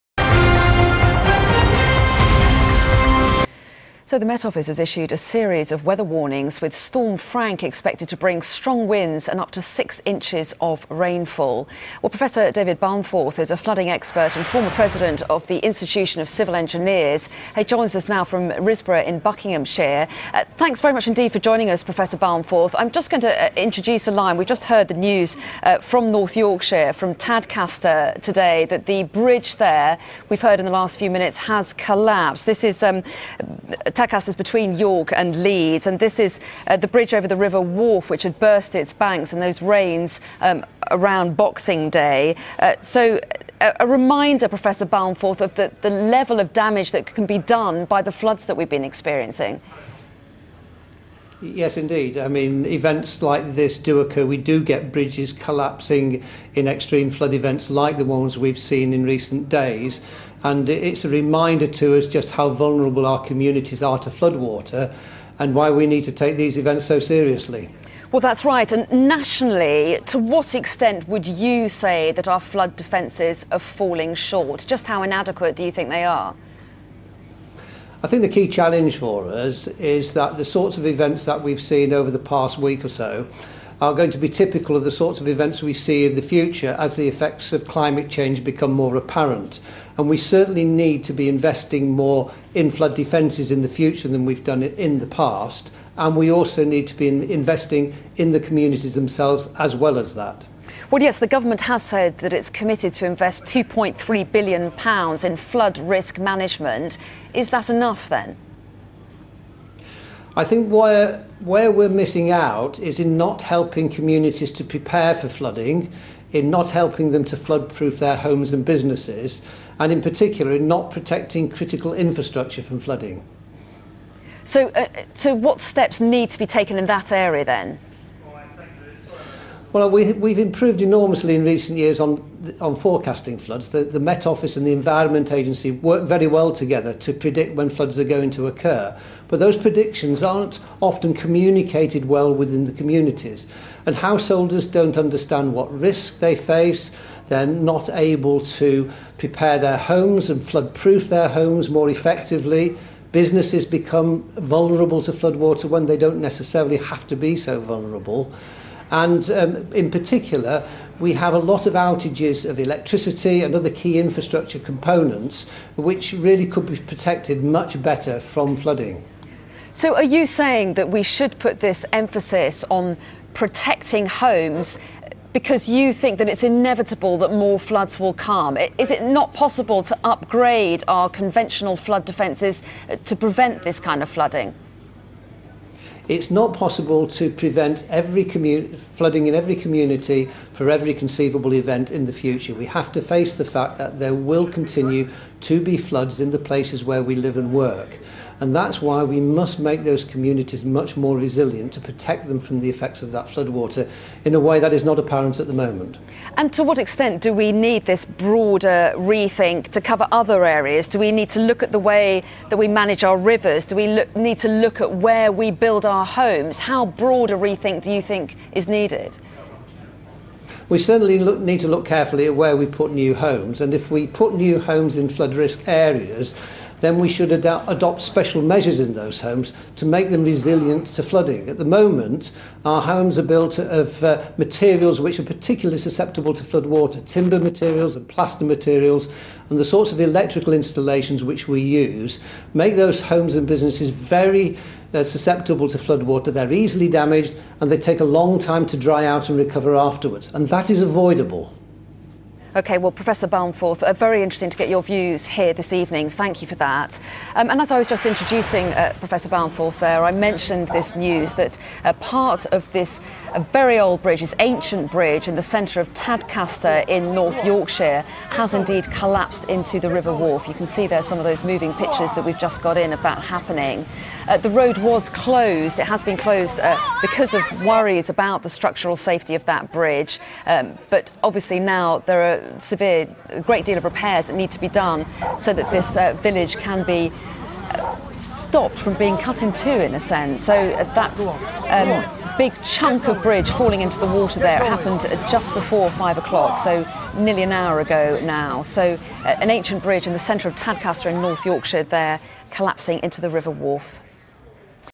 ICE flood expert Professor David Balmforth on Sky News
ICE’s past president Professor David Balmforth, was interviewed by Sky News on 29 December about the ongoing flood events across the UK, and the need for a more holistic approach to flood resilience.

Find out more about our work on flooding and explore our resources